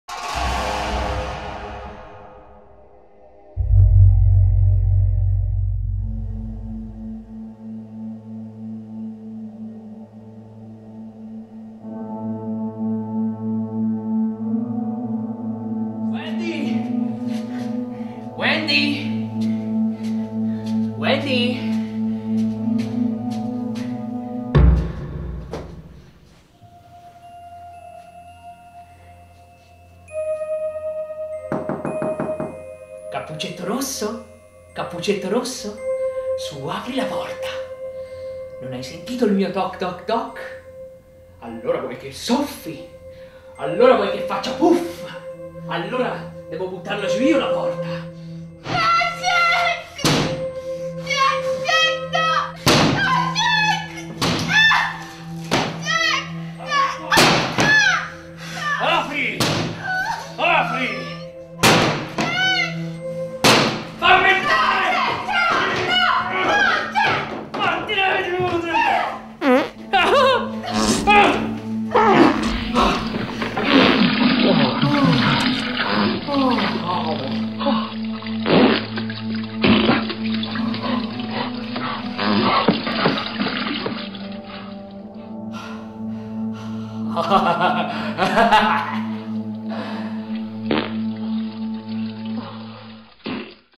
SCIAINING
L’originale di Stanley Kubrik mi ha sempre fatto cacare addosso dalla paura. Chi può scordare lo sguardo di Jack Torrence? Chi può dimenticare le due gemelline trucidate e la sagoma lugubre dell’Overlook che si staglia nella notte? Noi. Infatti l’abbiamo rifatto per riassumere in questo minuto e mezzo, tutto l’orrore possibile. Diciamo che noi di Grezzofilm abbiamo fatto quello che Kubrik avrebbe di sicuro voluto girare, ma non poteva, perché i tempi non erano maturi.

Curiosità:
La sequenza iniziale ha le stesse inquadrature dell’originale… (o, almeno così mi sembra a me).
Il mì babbo m’ha preso a calci la mia fiat Duna, per rappresaglia dopo che ho sfasciato una porta di casa sua per la scena.
Quel maglione rosso (Nicholson nel film aveva una giacchetta rosso-culo-di-bimbo che non son riuscito a trovare) me l’ha prestato una che lavora nel ristorante di mì padre e puzzava di fritto che levava il naso, ma io non gliel’ho mai reso e anzi, è diventato il pezzo forte del mio guardaroba
(estivo). Un comico (si fa per dire) di cui non faremo il nome ma diremo solo che si veste da pupazzone e fa le faccine in un noto programma tv, ce l’ha copiato paro paro un paio di anni fa, ma noi l’abbiamo fatto prima, ciccio (nel 2004)!

Interpreti:
Jack Torrence: Andrea Camerini
Wendy Torrnece: Stefania Tidda
Riprese: Un po’ io un po’ la Tidda, visto che quel giorno eravamo in 2.
Direttore della fografia: disperso in una rissa tra nani.
La porta: era di mì pa’.